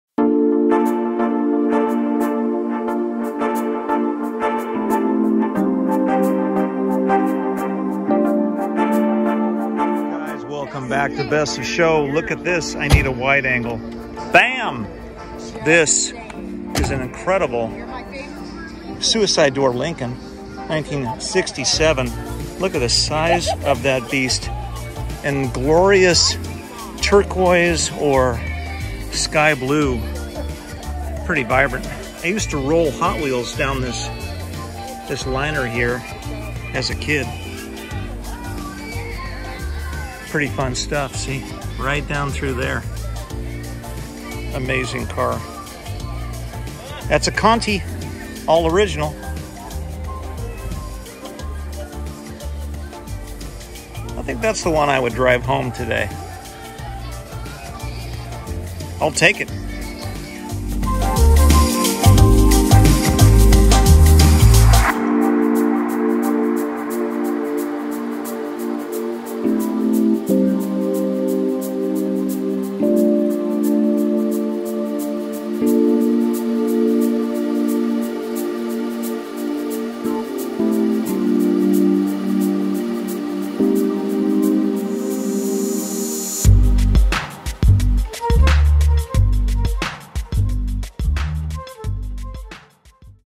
1967 LINCOLN CONTINENTAL! - Fireball Tim BEST OF SHOW
1967 LINCOLN CONTINENTAL! - Fireball Tim BEST OF SHOW
Cruisin' through San Marino, Fireball spots this stunning classic 1967 Lincoln Continental in Aqua...